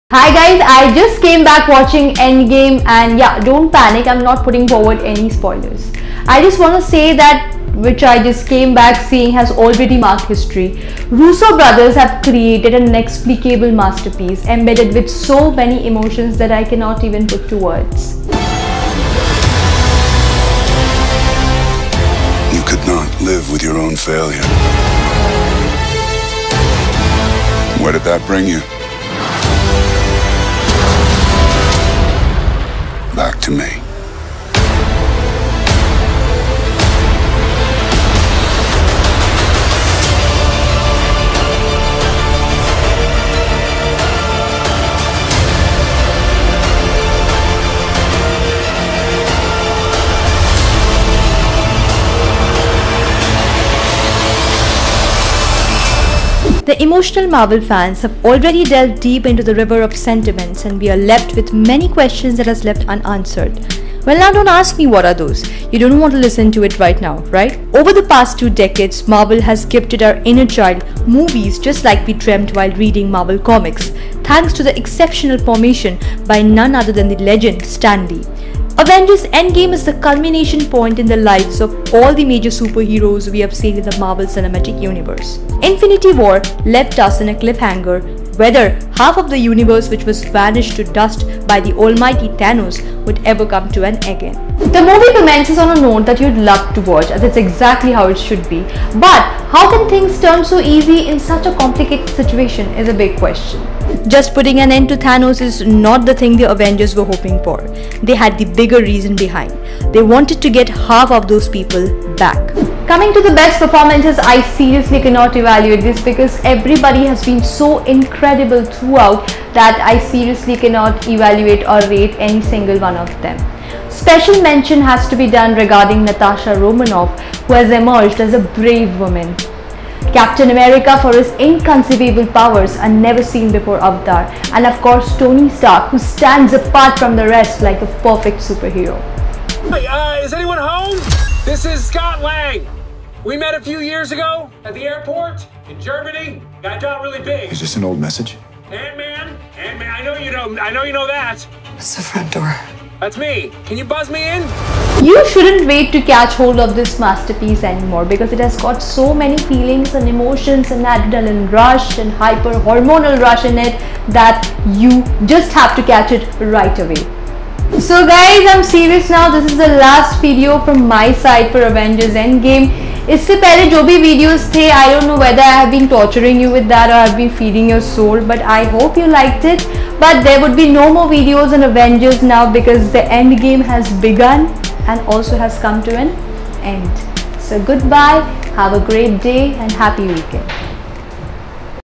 Avengers EndGame Movie Review | LopScoop
Let’s admit it. You aren’t looking for a review for Avengers Endgame . Coz if you are then this film isn’t for you anyway. Our love affair with the Marvel  11 years laters it all
Comes down to this fantastic finale